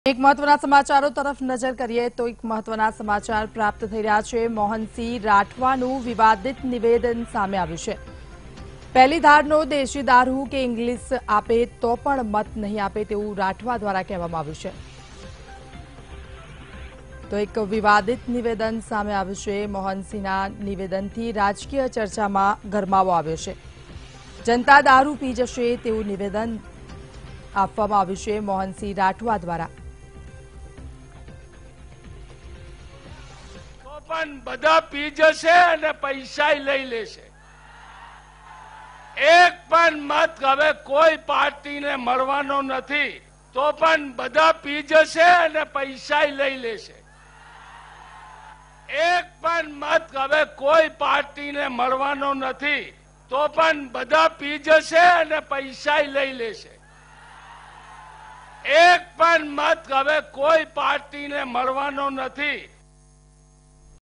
Voters will not vote even if BJP provides Bottles of Liquors: Congress MLA Mohan Rathwa | Vtv News
Voters will not give vote even if BJP provides Bottles of Liquors for Gujarat Vidhan Sabha Elections: Congress MLA Mohan Rathwa